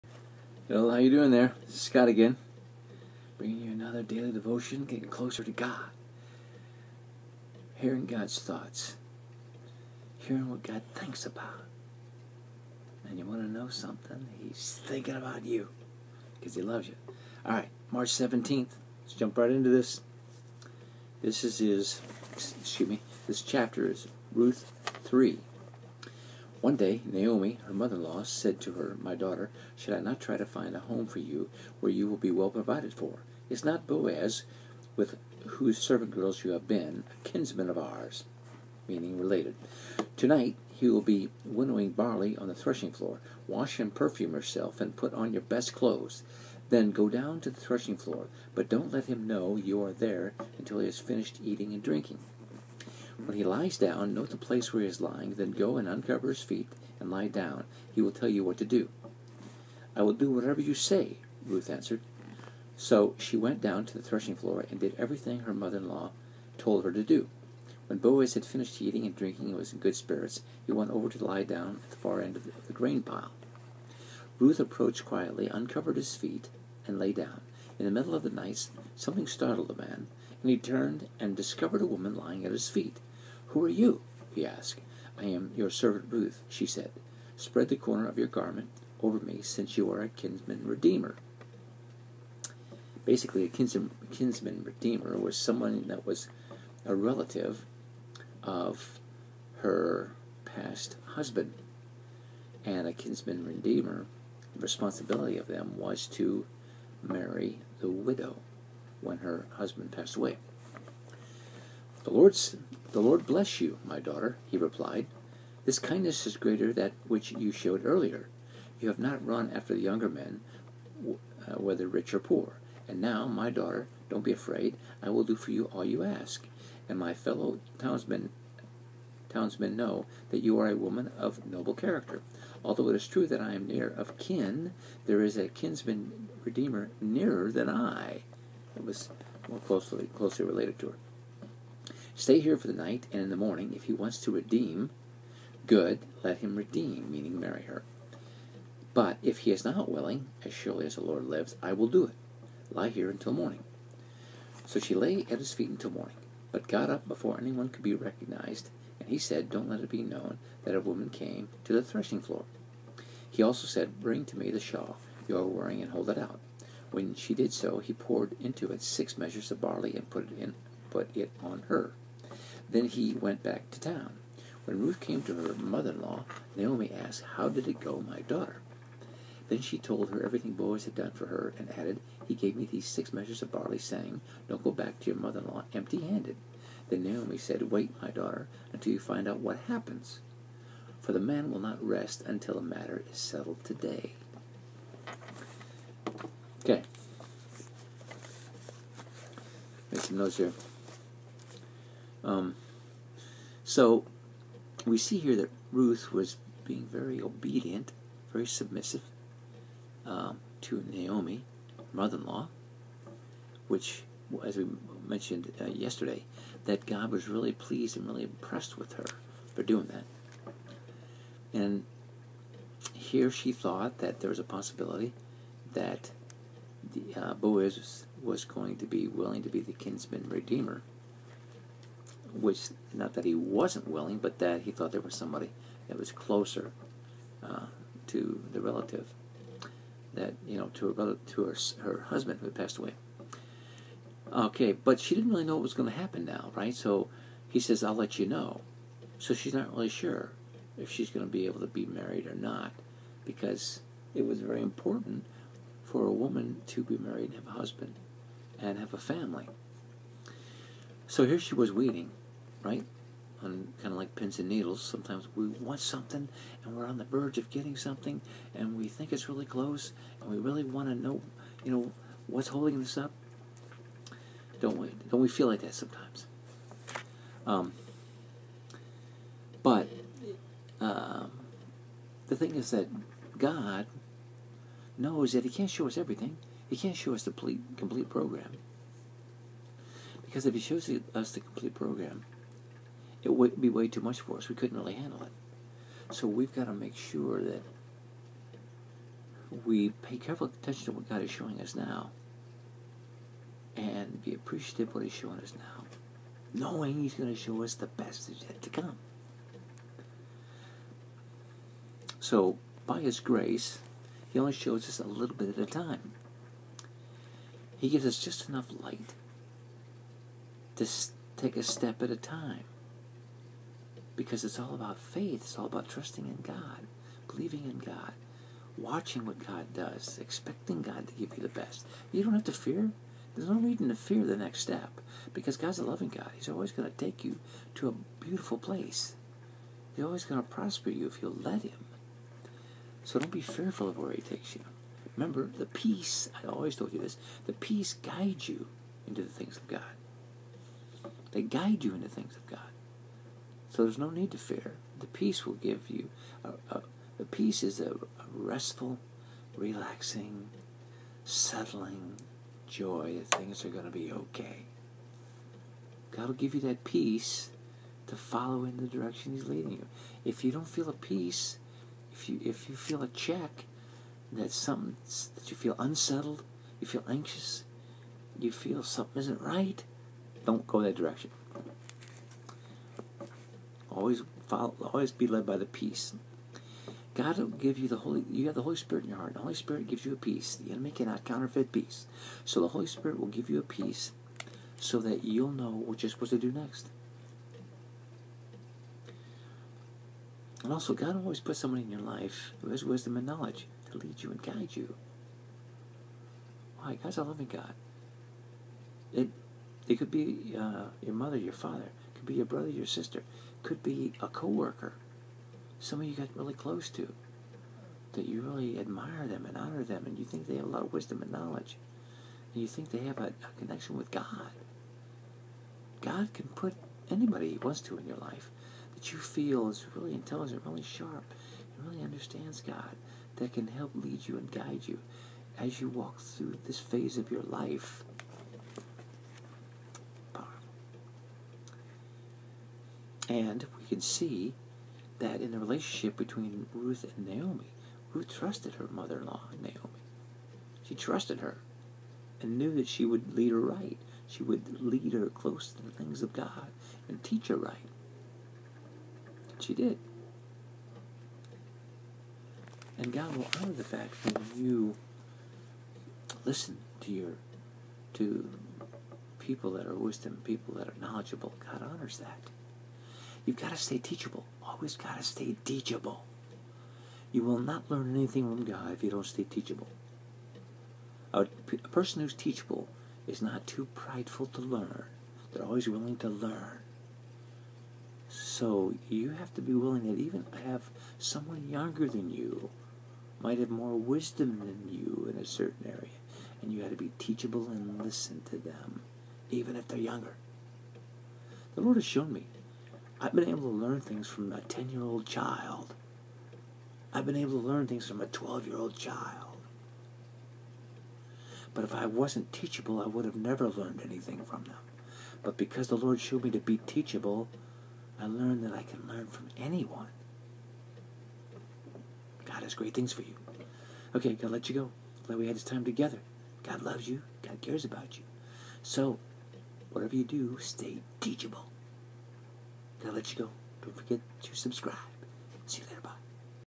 Daily Devotional - Trust His light for the day and know that Your Blessings are coming - 03/17/2014
Hello, Please join me in my daily devotional Vlog for the year 2014! I would like to encourage you with a closer walk with our Lord and Savior Jesus Christ. I'll be up loading a new video each day for this entire year.